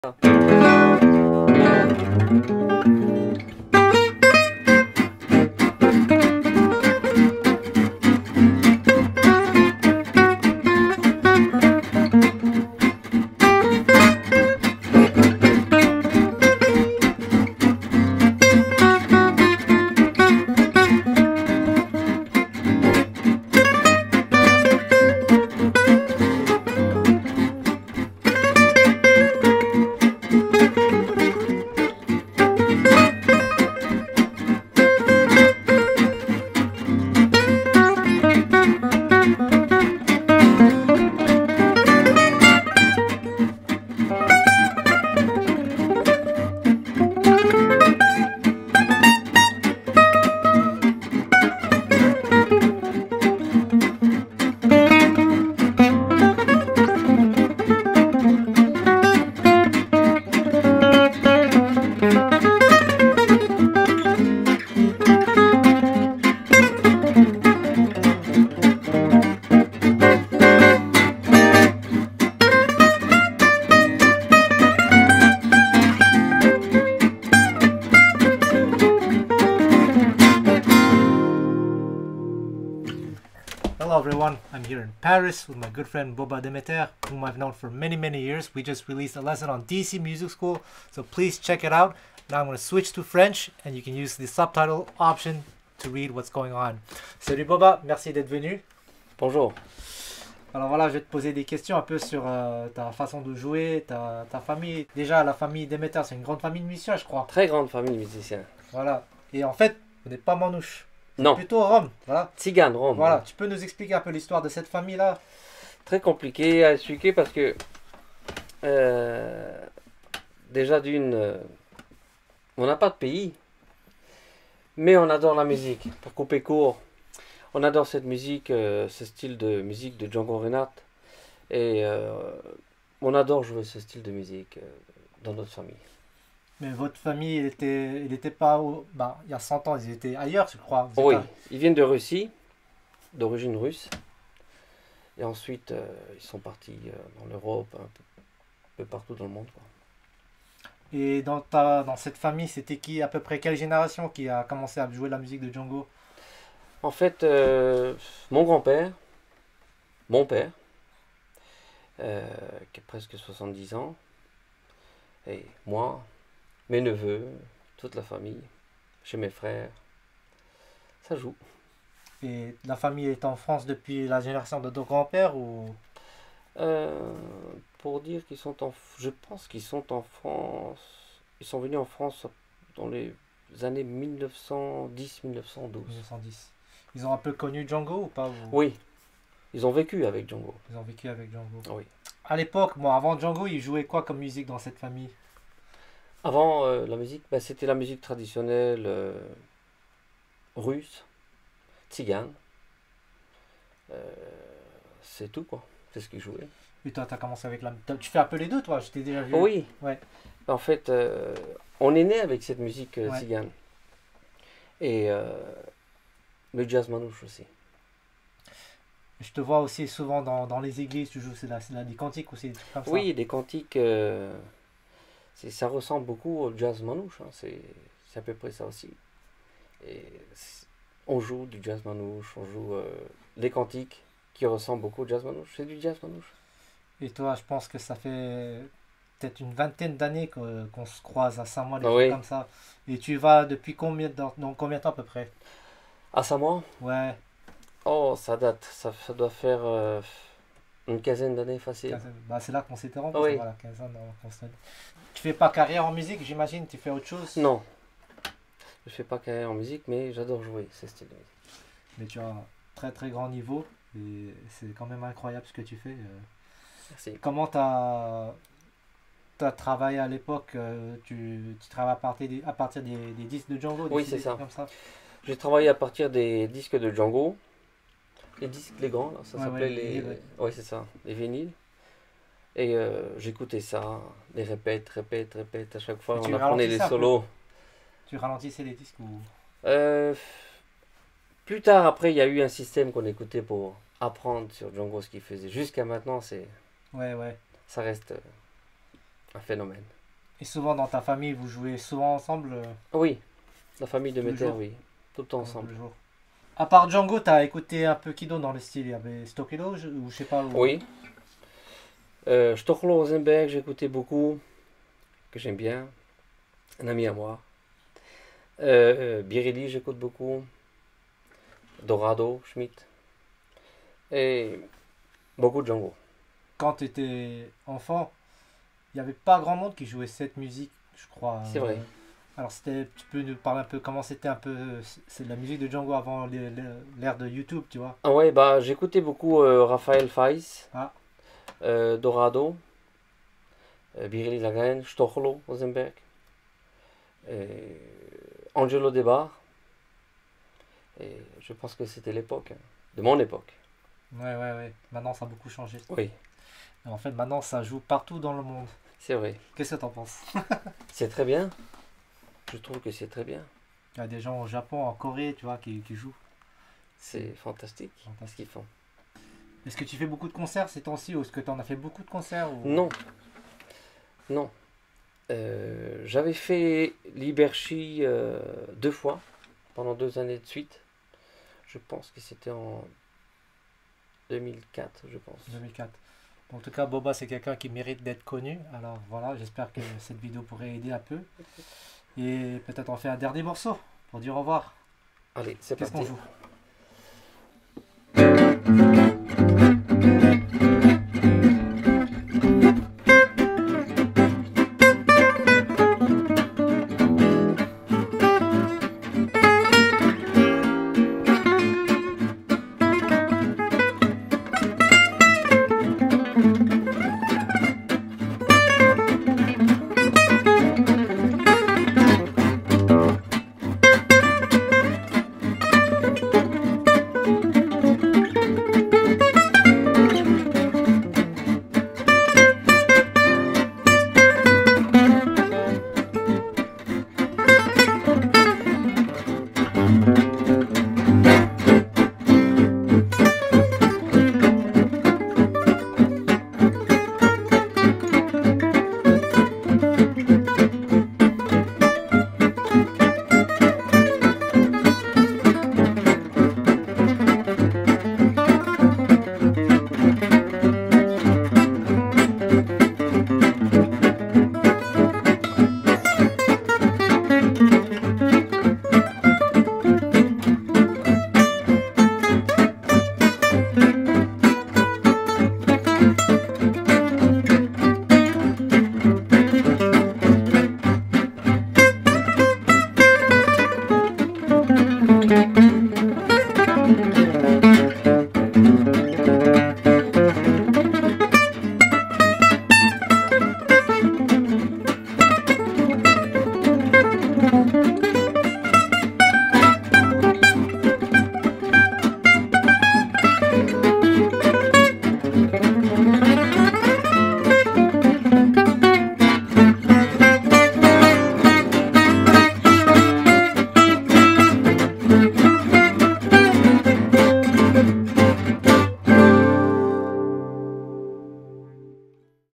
Boba Demeter - Phenomenal Gypsy Jazz Guitarist from A Legendary Russian Roma Family
My Gypsy Jazz Picking Book: